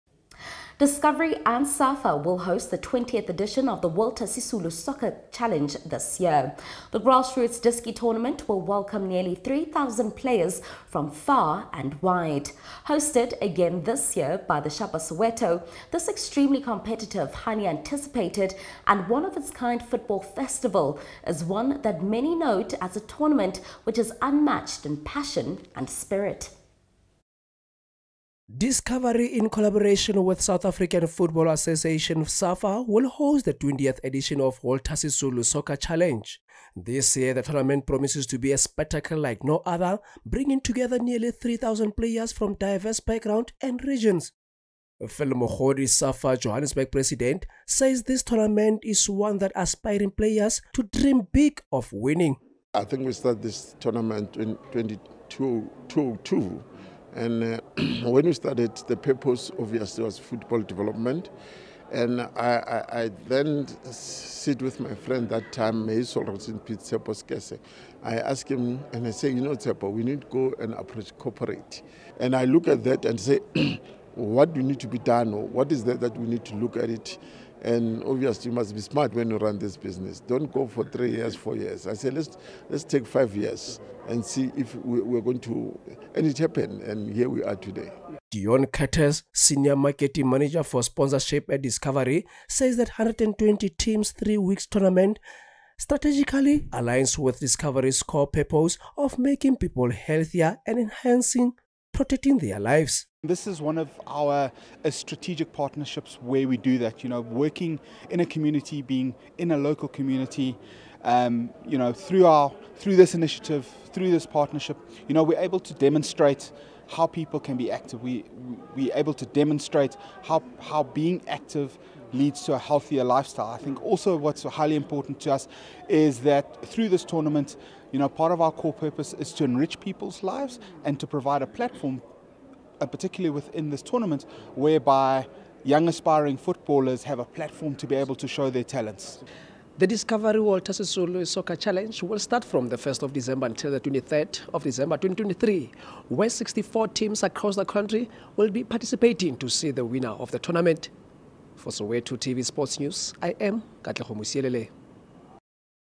SOWETO TV NEWS | 20th DISCOVERY WALTER SISULU SOCCER CHALLENGE LAUNCHED
Discovery and the South African Football Association launched the 20th edition of the Walter Sisulu Soccer Challenge that will held at Shapa Soweto#sowetotvsportnews